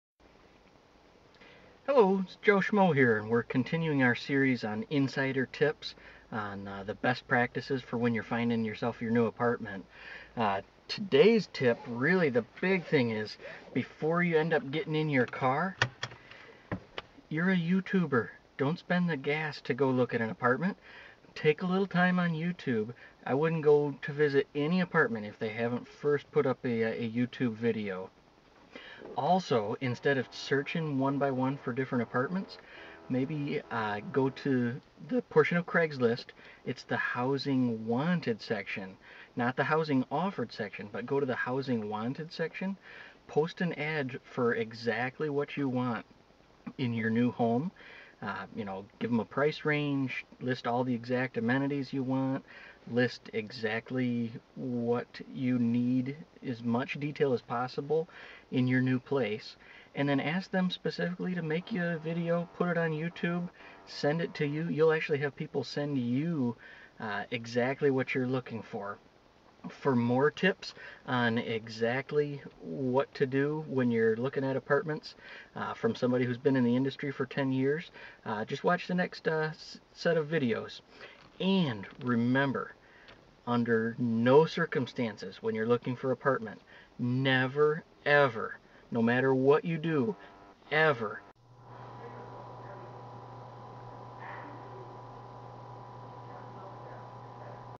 How to Rent an Apartment in Washington DC -- Insider Tips -- Video F
Finding and renting an apartment is easy, but finding and renting a good apartment with a good landlord where you will have a good experience is not as simple.  I hope this short video series will help you have a great renting experience!